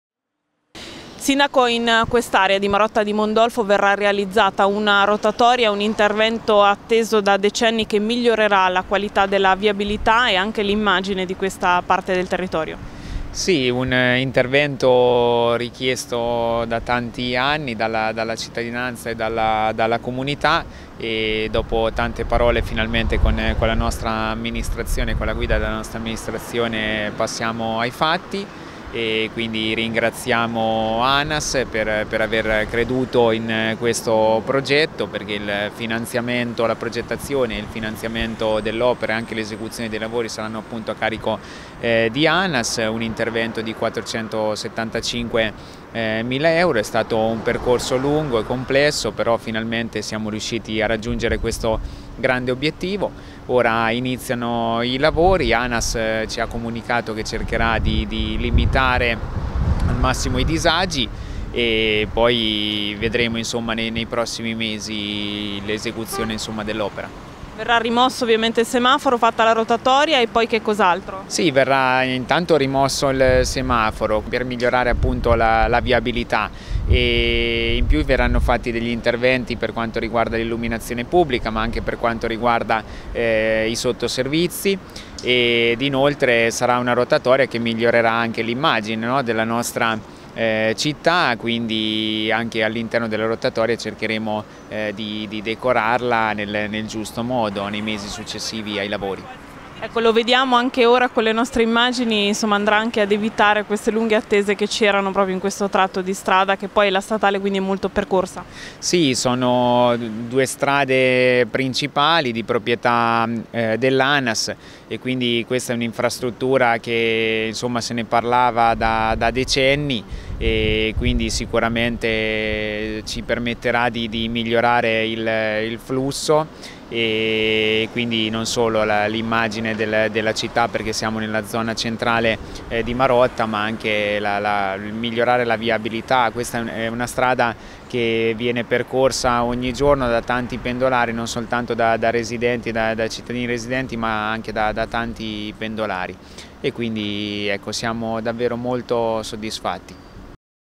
Marotta: al via il cantiere per la realizzazione della rotatoria tra l'Adriatica e la Pergolese
Al via a Marotta il cantiere Anas per la realizzazione della rotatoria tra la Statale Adriatica e la Pergolese